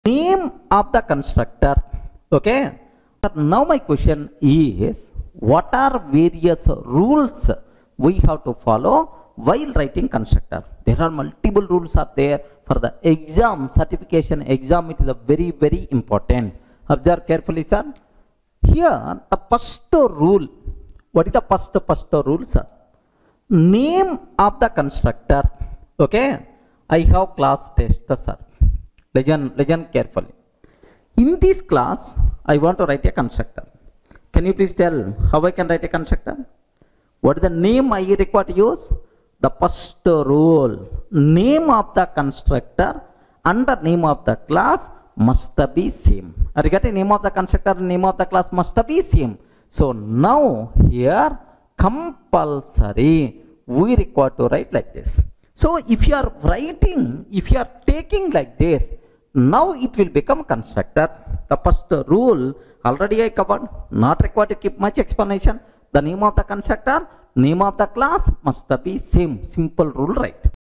Constructor Rules: Essential Java Coding Secrets Revealed
Writing constructors? Rule #1: The constructor name *must* match the class name. Simple, but crucial for certification. Get it right the first time!